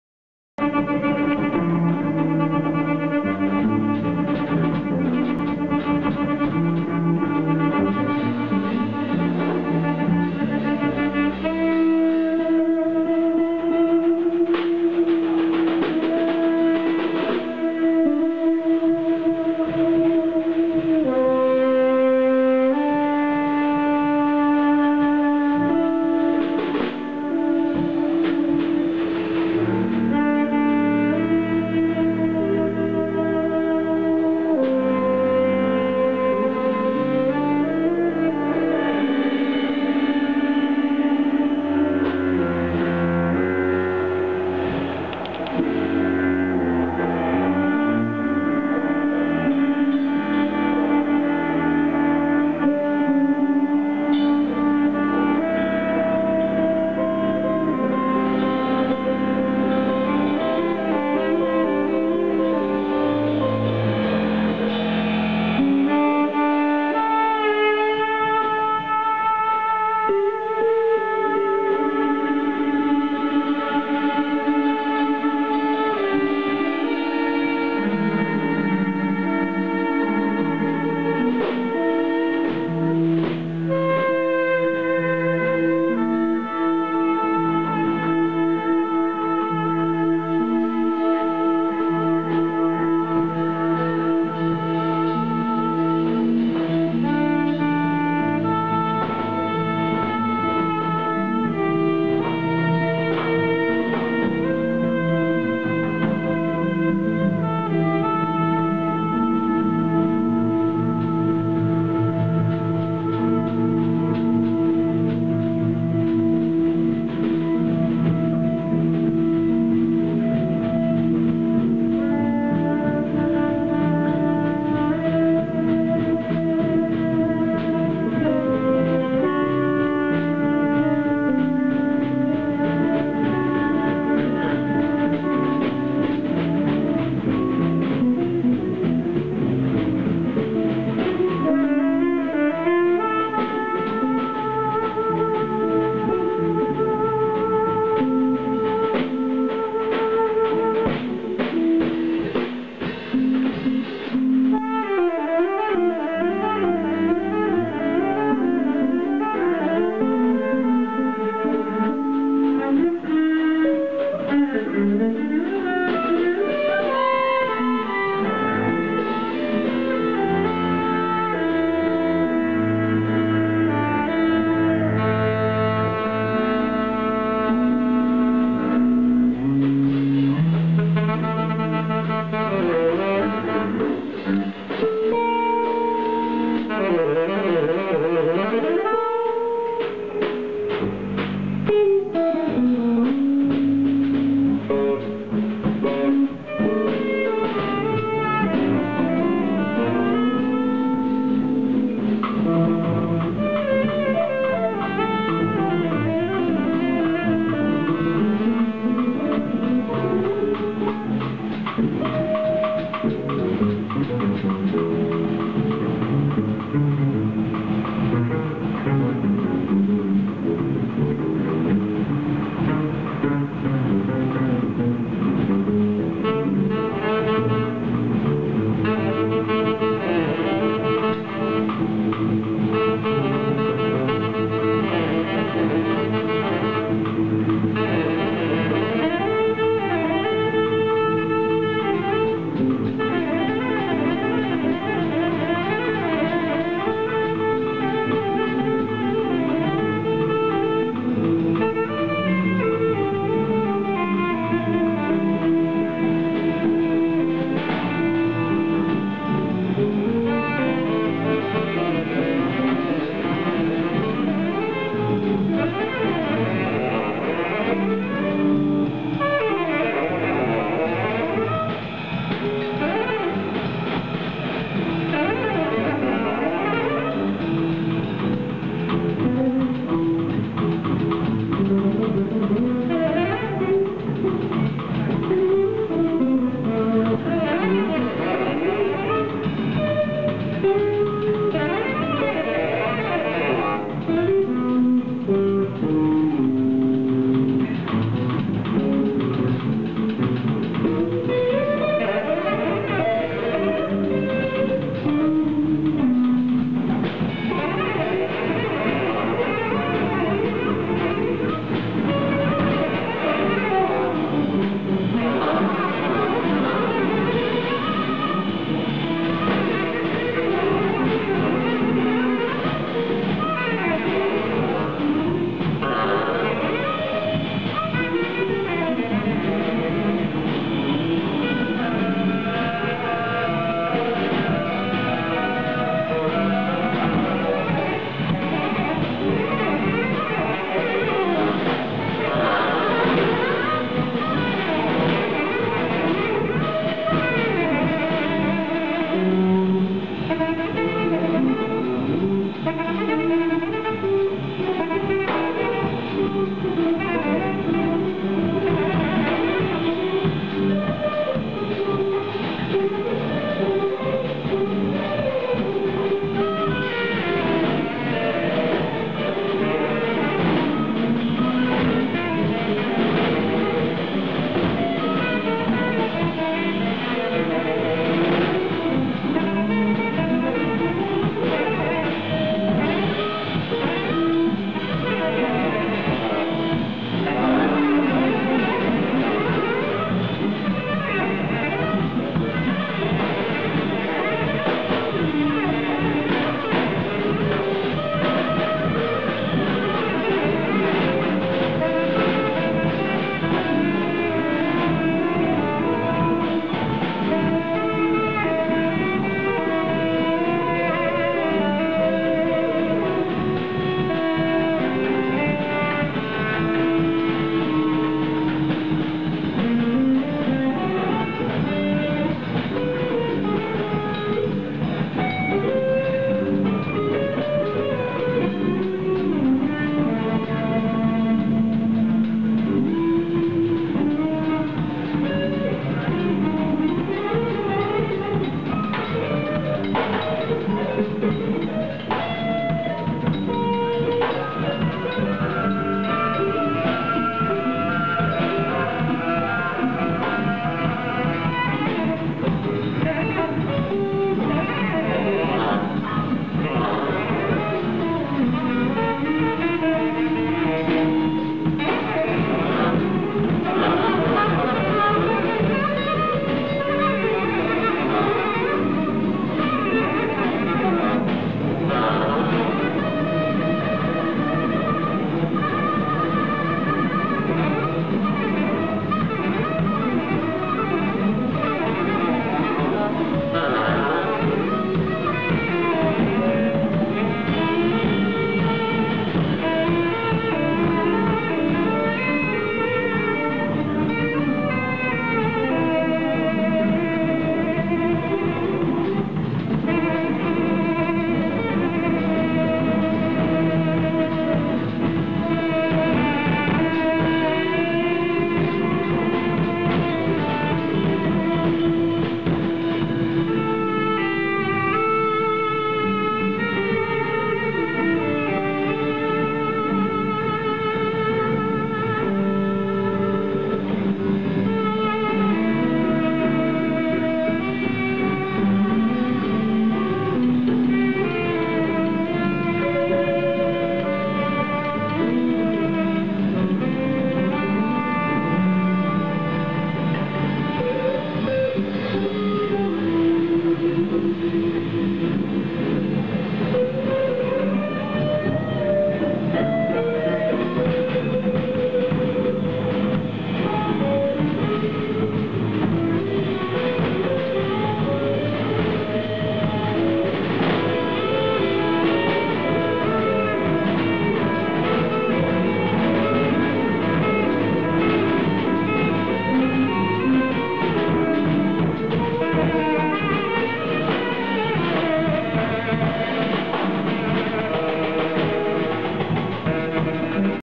DCIC at ArtDc (Complete)
DC Improvisers Collective at ArtDc November 21, 2009. We played in the gallery space becoming part of the exhibit. People flowed in and around us, some sat and watched intently, others continued to browse the art work and help themselves to refreshments. We were joined by Vattel Cherry on bass. Ben Azzara on drums, Mike Sebastian, saxes, Jon Matis, guitar. Thanks to all who came out.